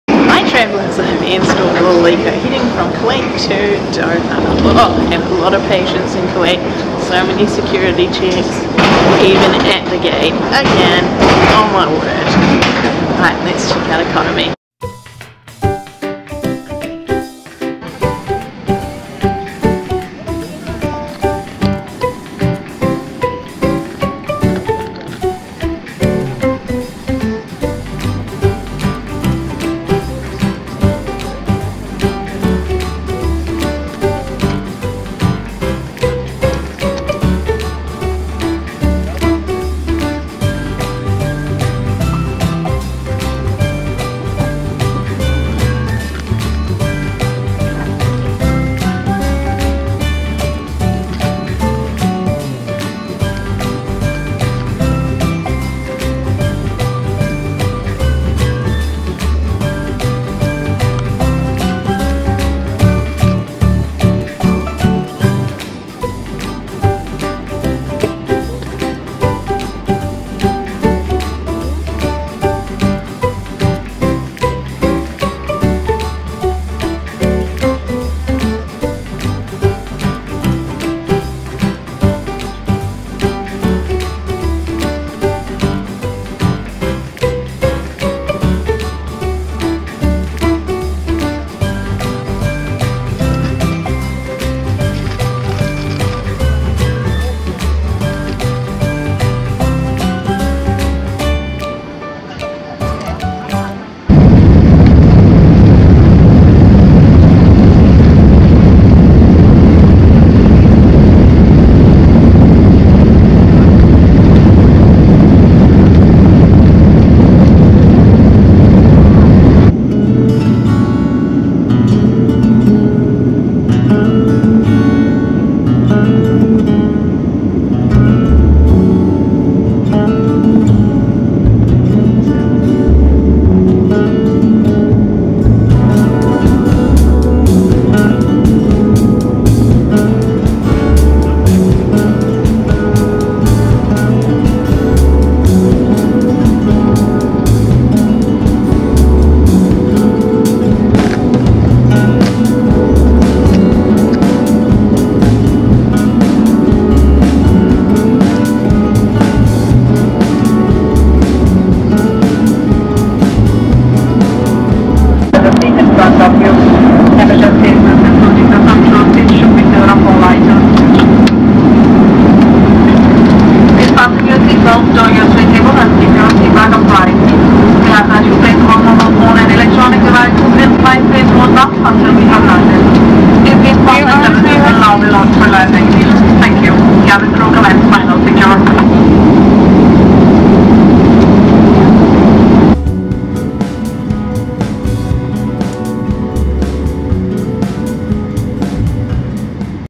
Qatar airways economy class Kuwait to Doha | QR1083
Hi travellers,
In this video I show inside my economy class flight with Qatar airways from Kuwait to Doha.  It's a very short flight of a little over an hour.

Flight QR1083 leaves Kuwait at 11.25pm and arrives in Doha at 12:50am.

I flew after work and it was a long day.

There is free wifi once in the air as its free for one hour.  So join as soon as it opens to make use of it.

They give a snack and as you can see in the video I am still munching away as landing procedures begin.

Qatar is part of the One World alliance so be sure your frequent flyer number is attached to the booking.  It was such a cheap flight that I did not earn points or status credits with Qantas, sigh.

I use Momondo travel search engine to book my flights: